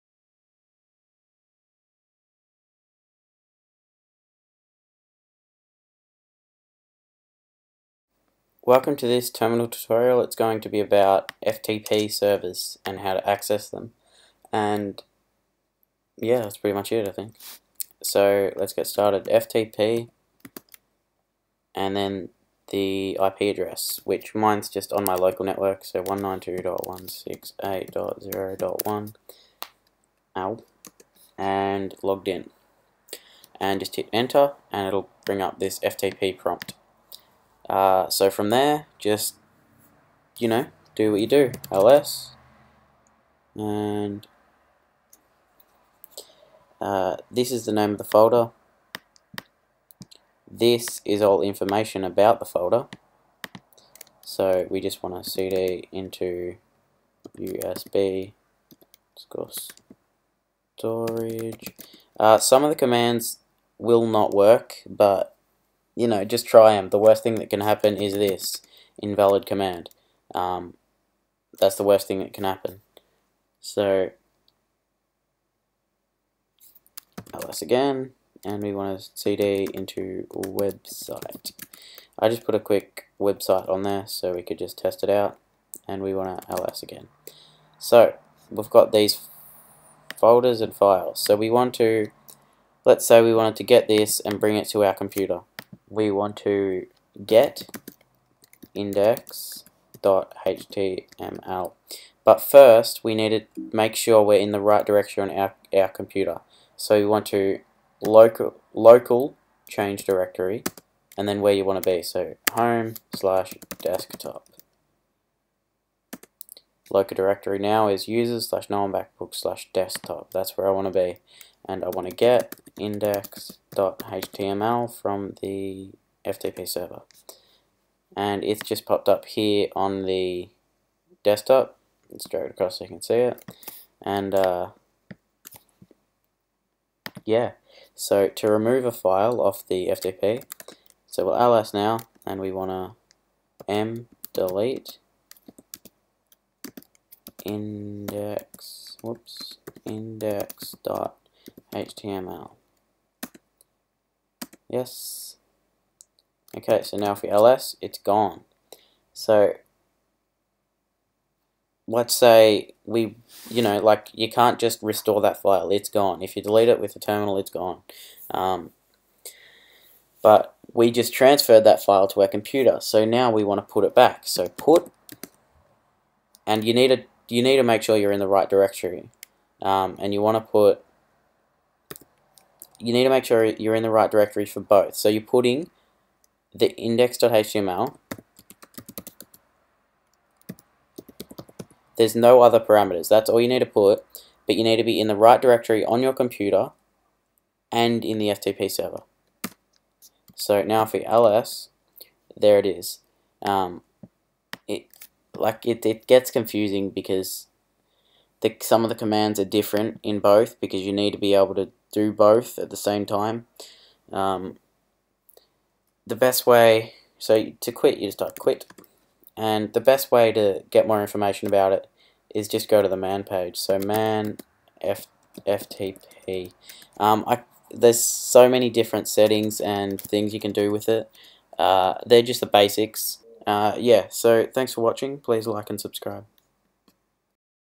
Terminal Lesson 20 - FTP
Welcome to lesson 20! In this lesson we will learn the basics of FTP. Also let us know if there are any tutorials you would like to see and we will do out best to provide them for you.

The commands we used in this tutorial:
ftp
ls
cd
lcd
mdelete